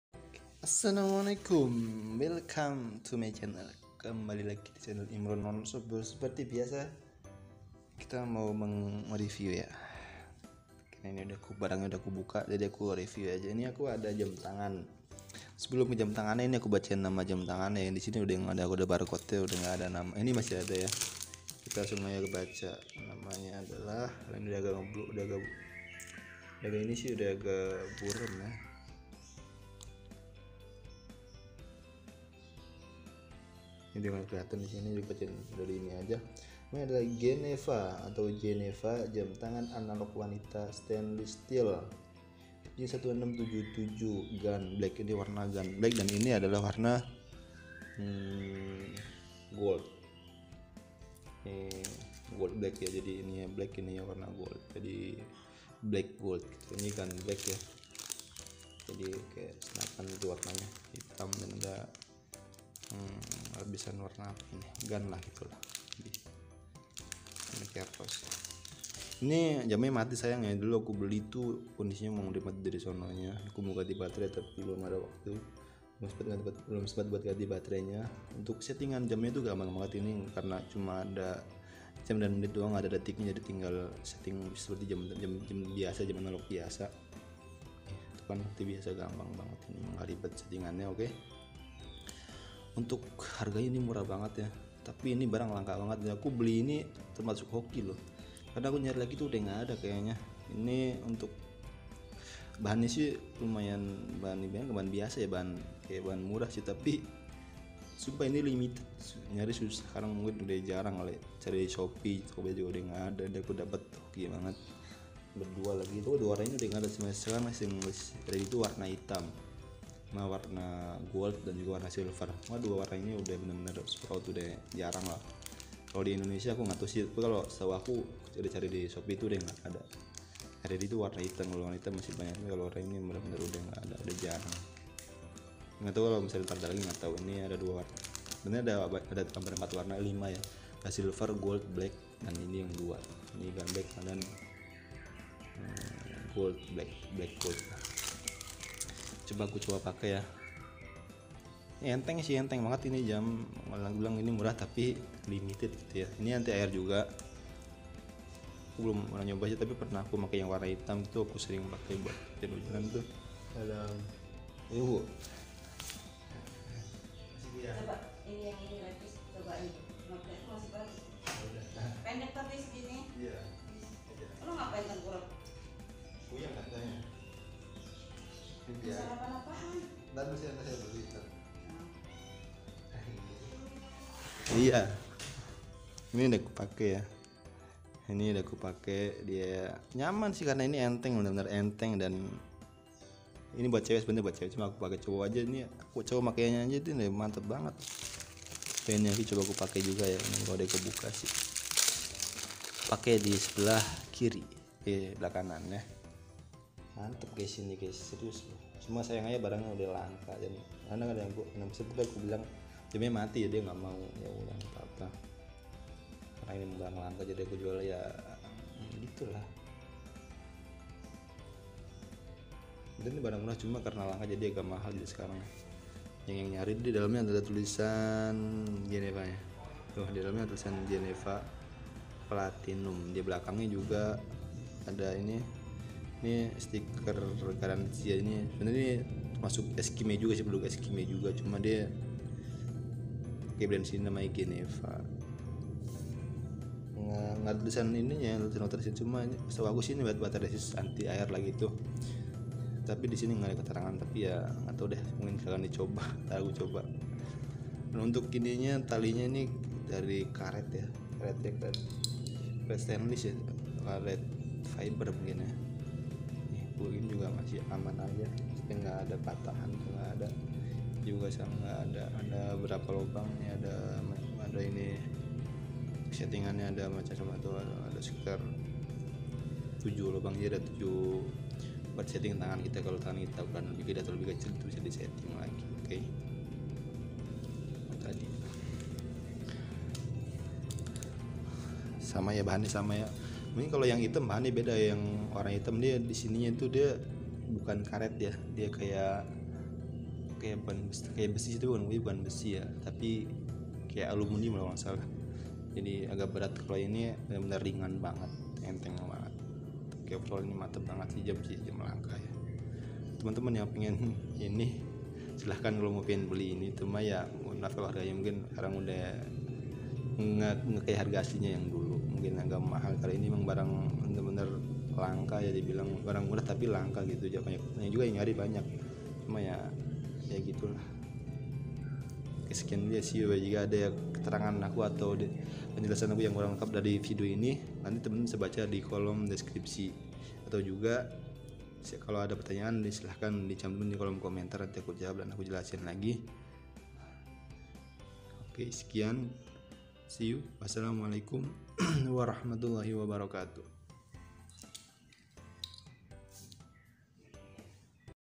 GENEVA Jam Tangan Analog Pria - 6209 | Rose Gold & Gun Black
Informasi Produk GENEVA Jam Tangan Analog Pria - 6209
Jam tangan ini hadir dengan interface penunjuk waktu analog yang unik dan stylish. Bagian tali terbuat dari bahan material stainless steel dengan warna yang menarik, menambahkan kesan modis dan elegan di dalam aktivitas sehari-hari.

Fitur
Antarmuka Analog yang Unik
Jam tangan ini hadir dengan interface penunjuk waktu analog yang unik dan stylish. Sangat cocok untuk melengkapi gaya berbusana Anda agar terlihat lebih keren.

Tahan Air 30 m
Jam tangan ini memiliki fitur sertifikasi waterproof hingga 30 m. Tidak perlu khawatir jam tangan akan rusak jika terkena air hujan.

Bahan Berkualitas Tinggi
Bagian strap jam tangan ini terbuat dari bahan stainless steel yang berkualitas, lentur dan tahan untuk penggunaan dalam jangka waktu lama.

Kelengkapan Produk
Rincian yang Anda dapatkan untuk pembelian produk ini:
1 x GENEVA Jam Tangan Analog Pria - 6209
Spesifikasi GENEVA Jam Tangan Analog Pria - 6209
Diameter Jam
40 mm
Jenis Kelamin
Pria
Material
Stainless Steel
Dimensi
Panjang Strap: 230 mm
Lebar Strap: 20 mm